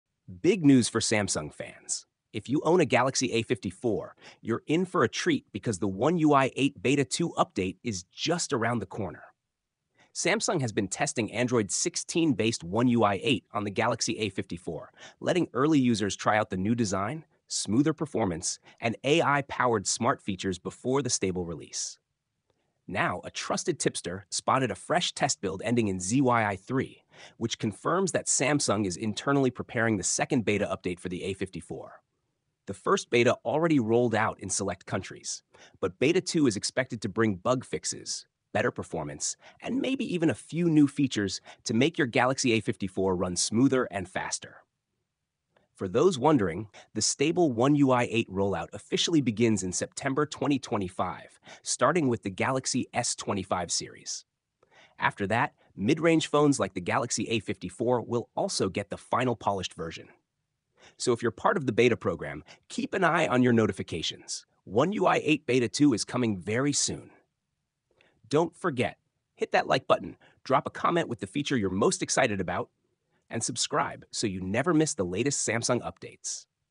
Samsung One UI 8 Surpirse Update on Galaxy A54
Samsung One UI 8 Surpirse Update on Galaxy A54 / Samsung Galaxy A54 One UI 8 Beta 2 Update

Samsung Galaxy A54 users are about to get the One UI 8 Beta 2 update, based on Android 16. The One UI Beta Program allows Galaxy A54 owners to test new features early and share feedback with Samsung. The upcoming Galaxy A54 One UI 8 Beta 2 build, spotted with version number ZYI3, suggests that Samsung is internally testing the update and will roll it out soon.

This new beta update is expected to bring bug fixes, performance improvements, and possibly new features that will enhance the Galaxy A54 experience. The first One UI 8 beta was released in select countries, and now Beta 2 is coming next.

Samsung has officially confirmed that the stable One UI 8 update will begin rolling out in September 2025, starting with the Galaxy S25 series, before reaching more Galaxy devices, including the A54. Until then, Galaxy A54 users in the One UI Beta Program can enjoy testing the latest features before anyone else.